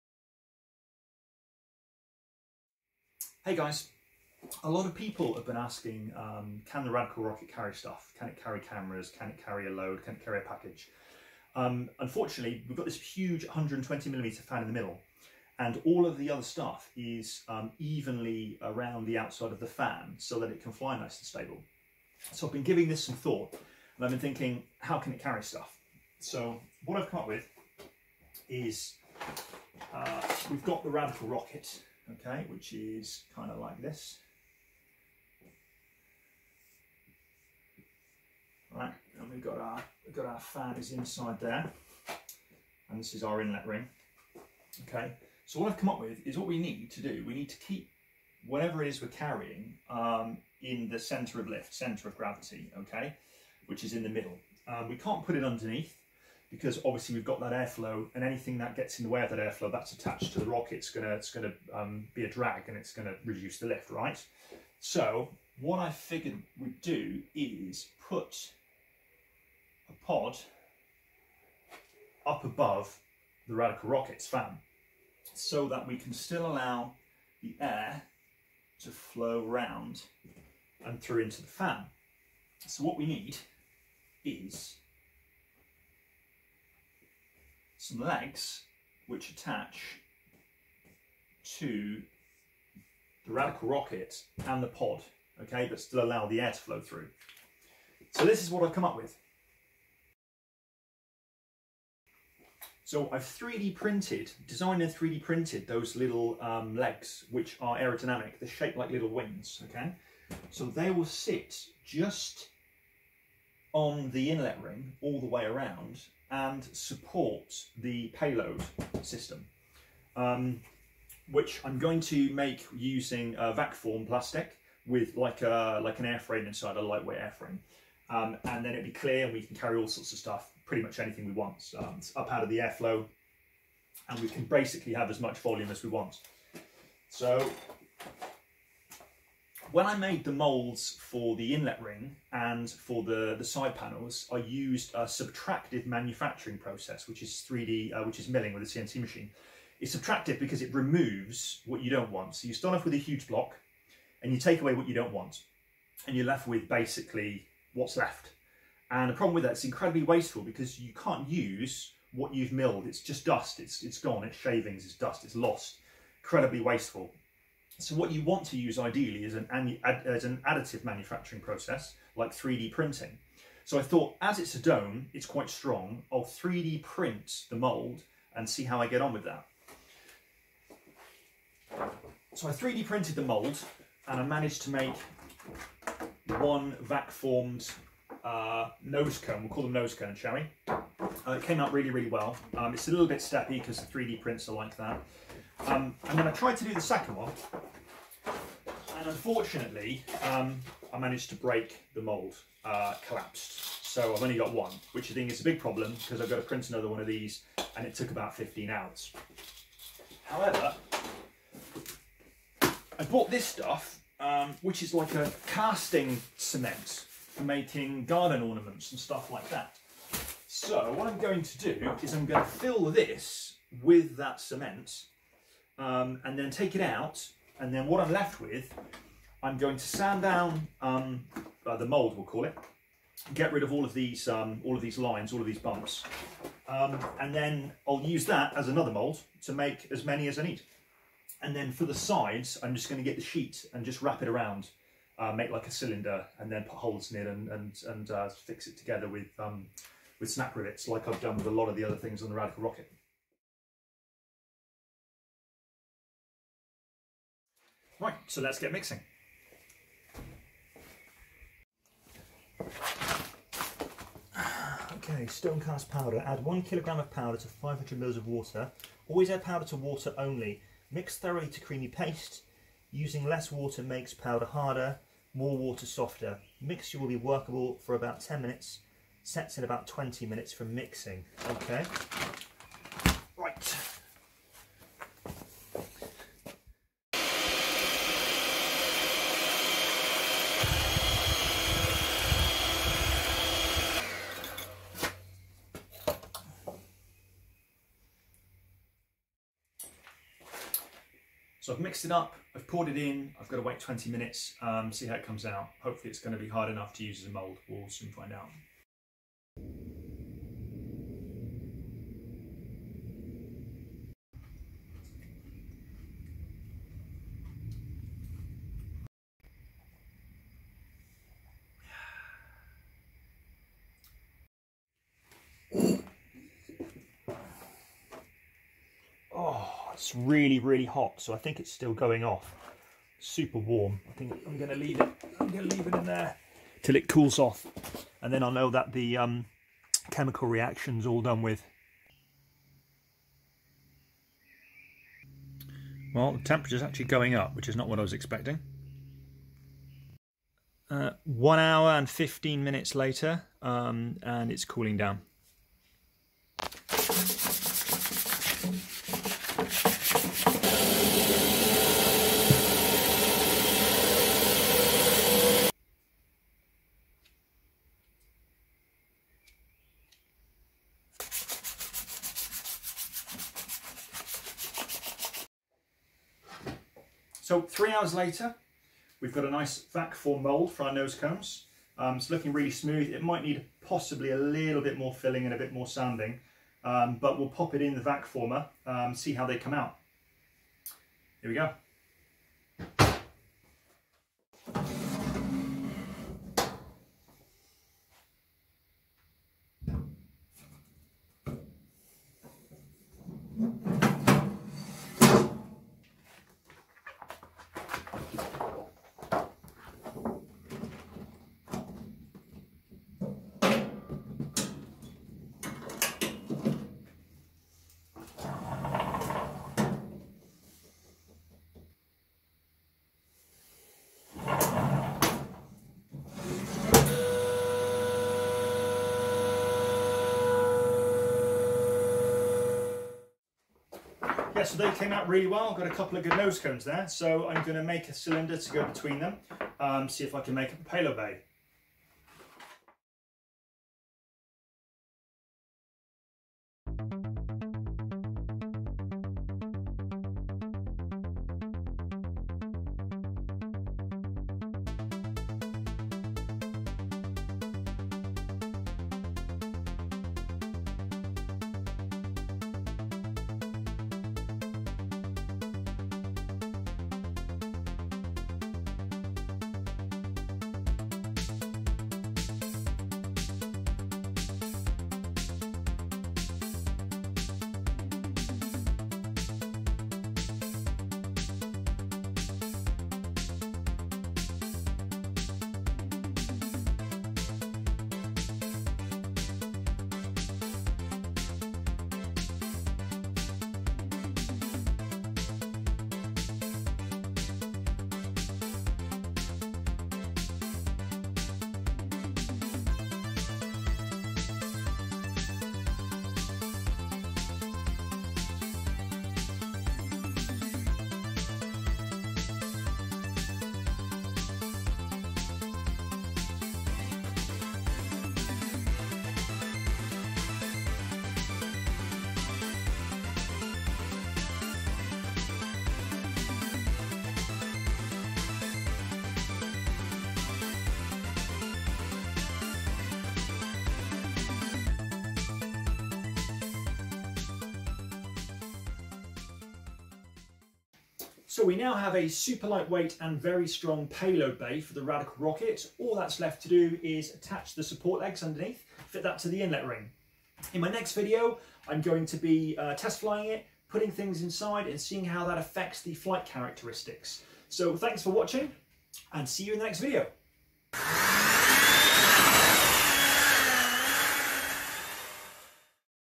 Payload System
People have asked if the Radical Rocket is able to carry things. In this video I design and produce a system that will increase the Radical Rockets capabilities.